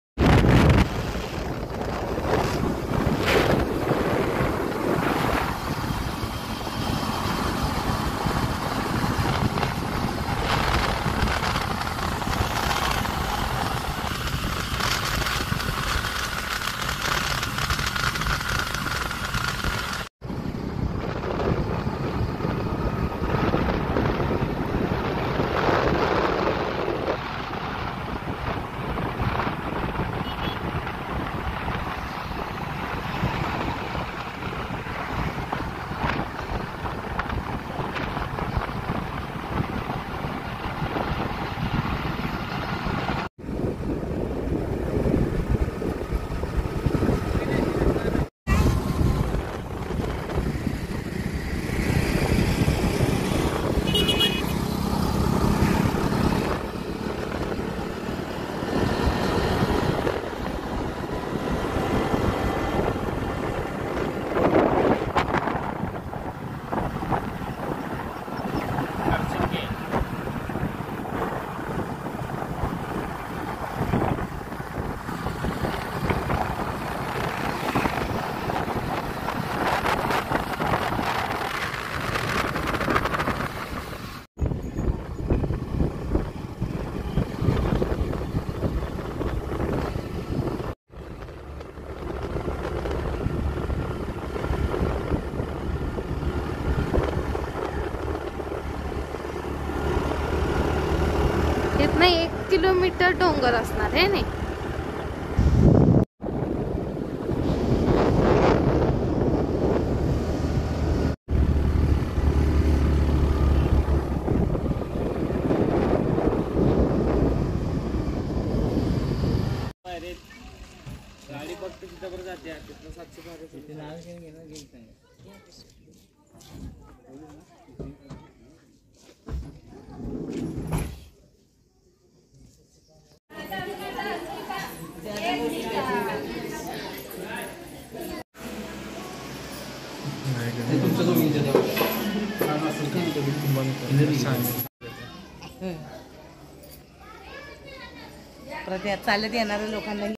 औंध, यमाई देवी दर्शन, सुंदर आकर्षित देवीचे रूप
सांगली ते औंध टू व्हीलर ने प्रवास. अतिशय सुंदर प्रवास झाला. खूप वर्षांनी देवी आई चे आमच्या कुटुंबांनी दर्शन घेतले. देवी आई असेच आमच्या कुटुंबाचे रक्षण कर.